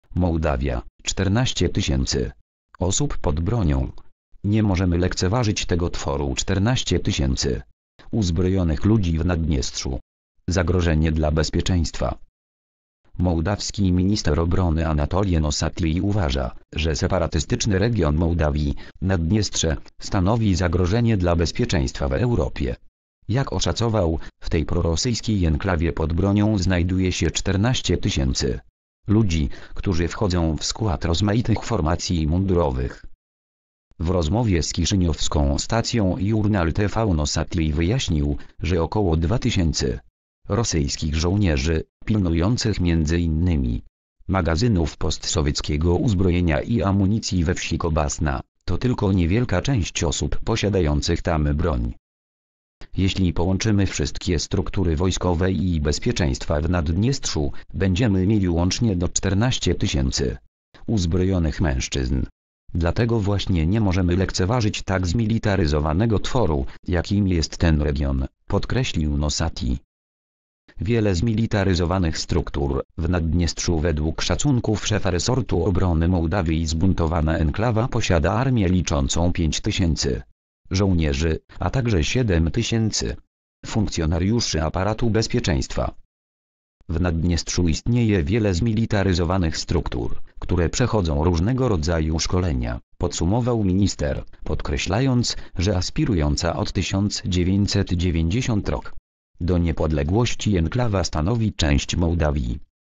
Mołdawia: 14 tys. osób pod bronią. "Nie możemy lekceważyć tego tworu"
14 tys. uzbrojonych ludzi w Naddniestrzu. "Zagrożenie dla bezpieczeństwa"

  (fot. iStock/soleg)